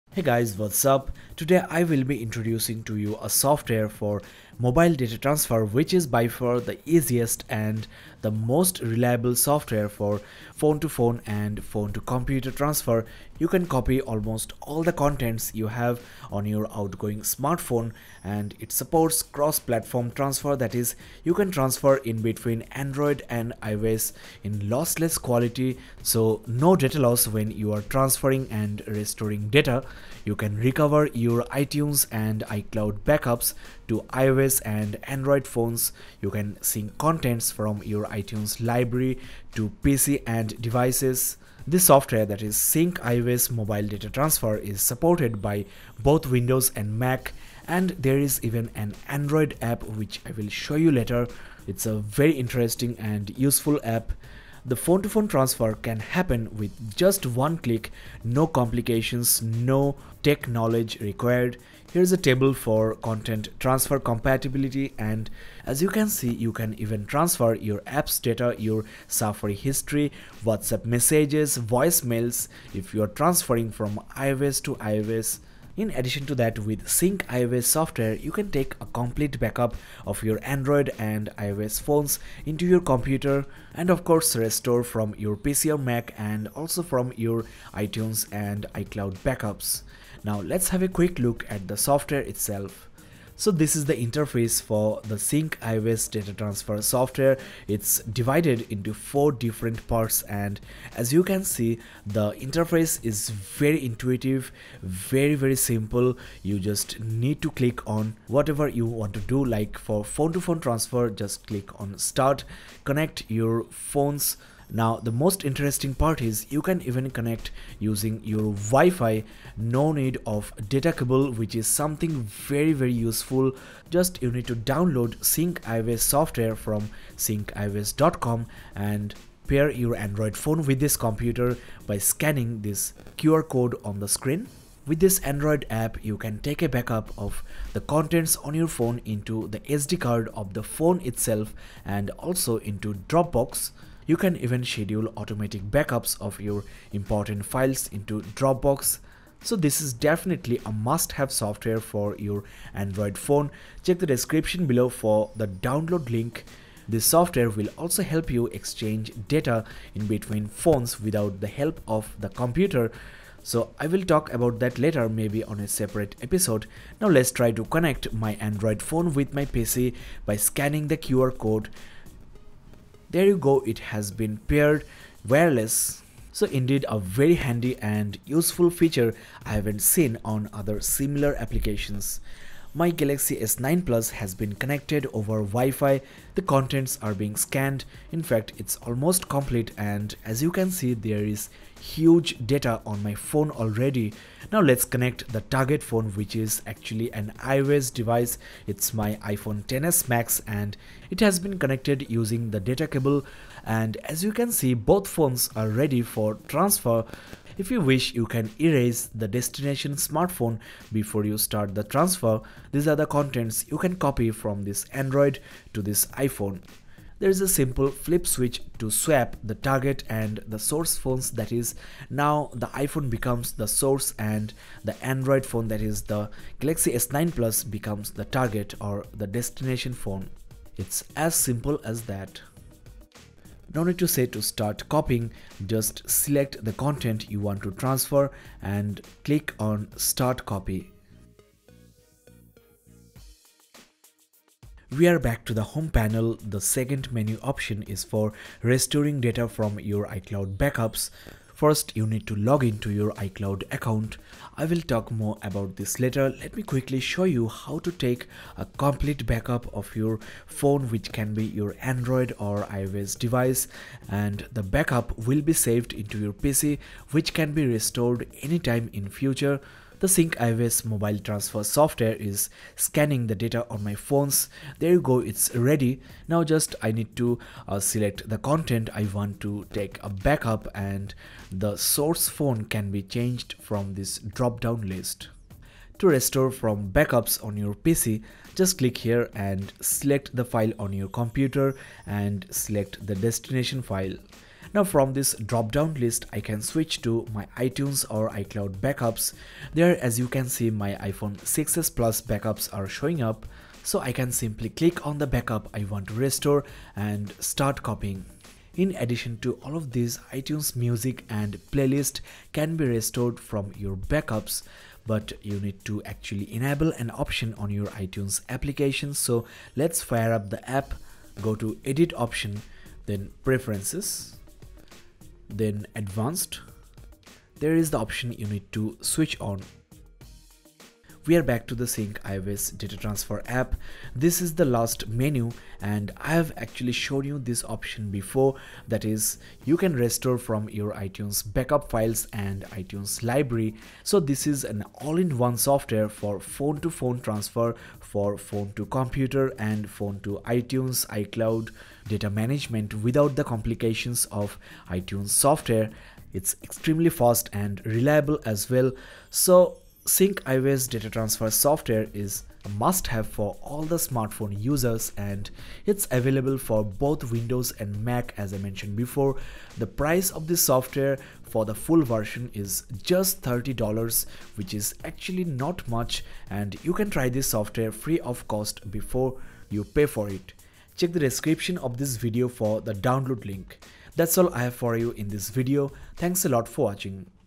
[FREE] How to Transfer Data From Phone to Phone (Android/iPhones) and PC? [Easy Way!]
Introducing Syncios Mobile Data Transfer software for your Windows and Mac - the best mobile data transfer solution for Android and iOS / iPhones. It's very simple, fast, reliable and doesn't require any technical knowledge. In addition to that, you can take a backup of your smartphone into your computer and restore anytime to the same or new device. Restoring of iCloud and iTunes backups can be done as well with this Syncios Data Transfer application.